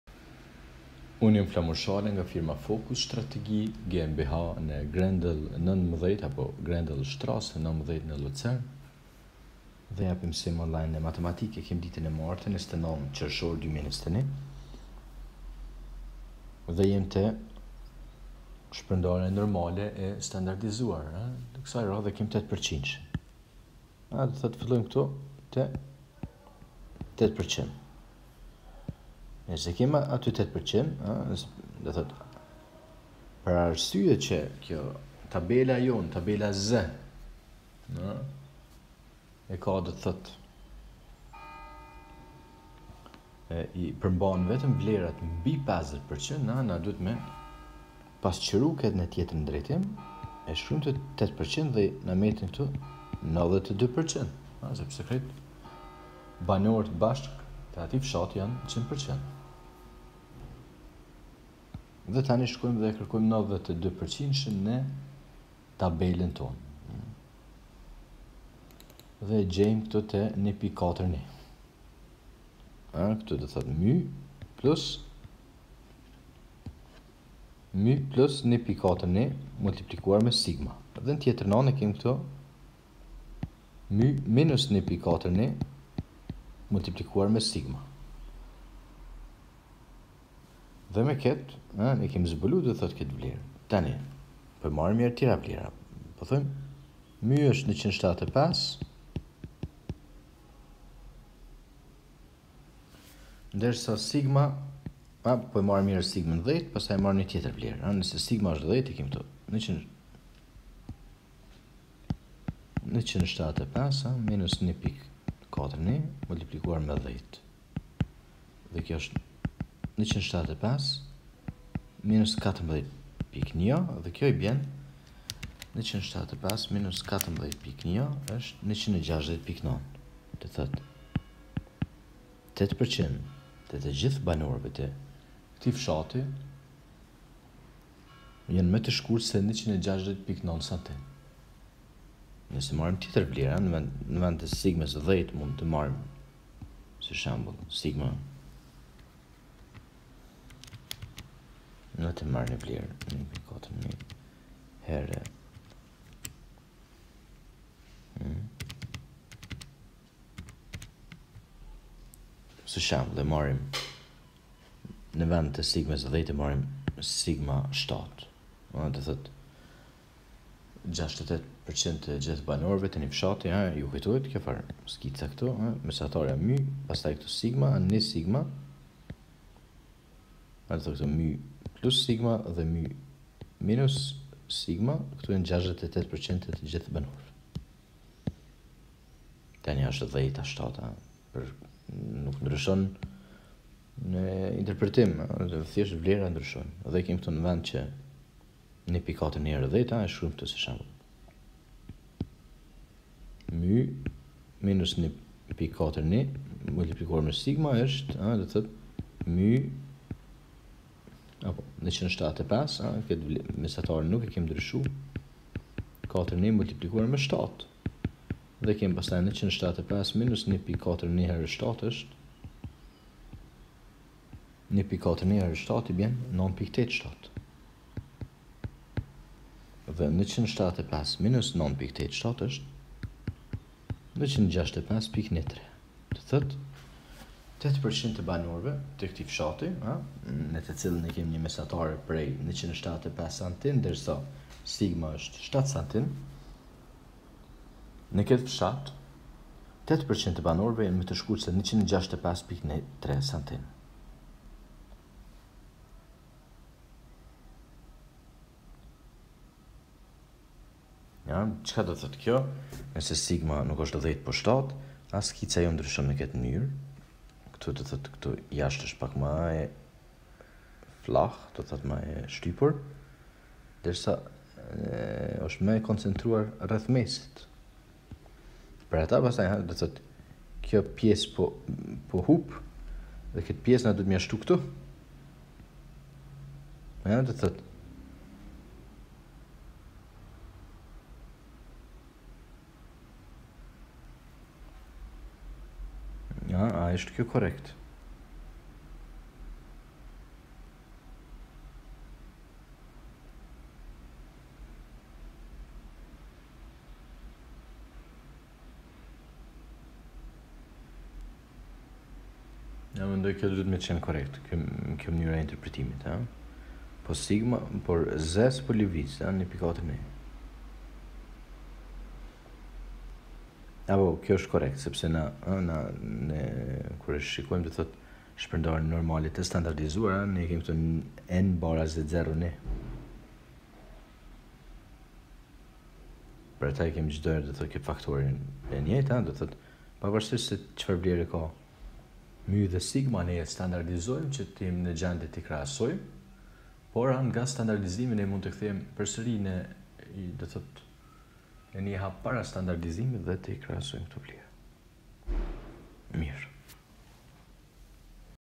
Matematika në Luzern | Statistika 141 | Shpërndarja normale e standardizuar z N(0;1) | Flamur Shala
Matematikë spjeguar nga Flamur Shala (Fokus Strategie GmbH, Grendel(strasse) 19, 6004 Luzern, 5.

Pergaditje per matura, universitet dhe punë.

Skype: flamur1717